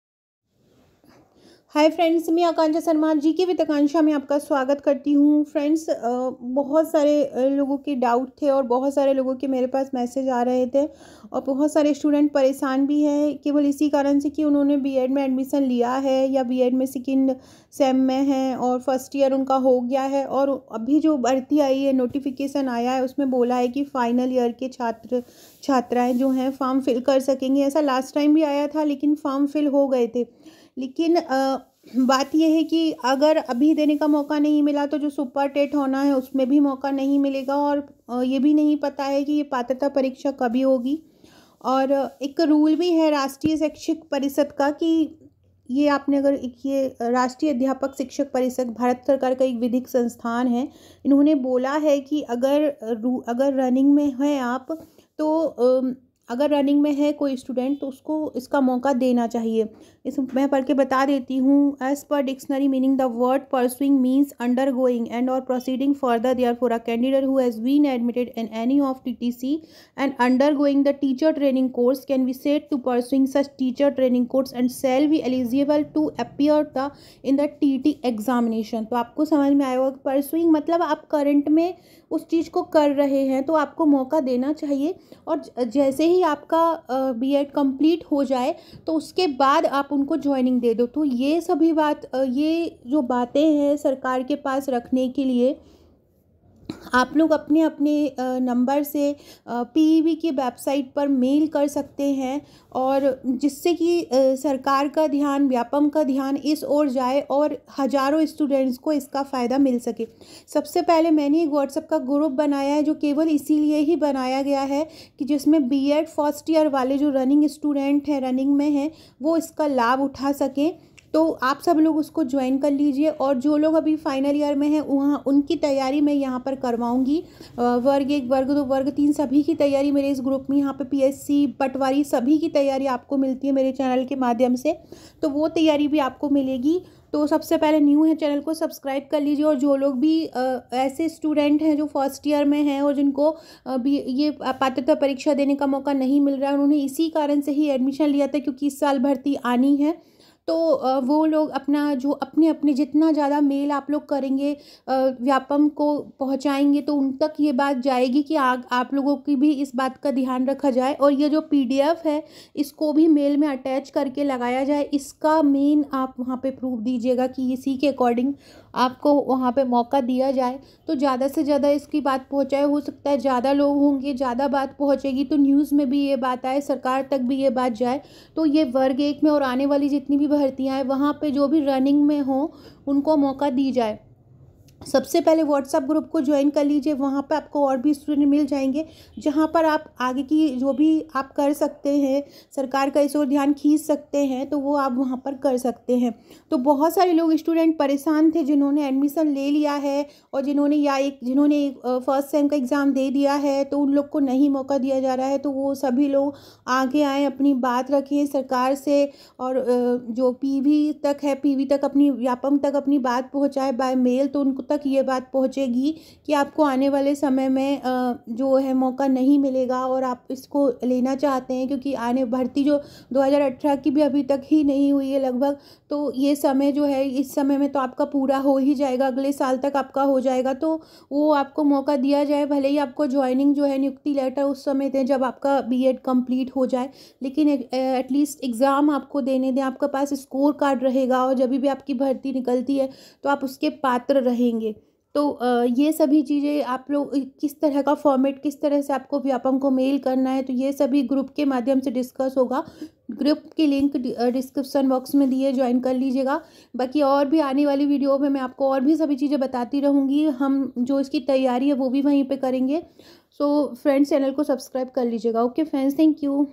mptet,verg-1, संविदा वर्ग 1 में B.Ed फर्स्ट ईयर वालों को भी मौका दिया जाए#bedrunning,bed first year
जो लोग भी फर्स्ट ईयर में है नीचे दिए ग्रुप से जुड़ जाएं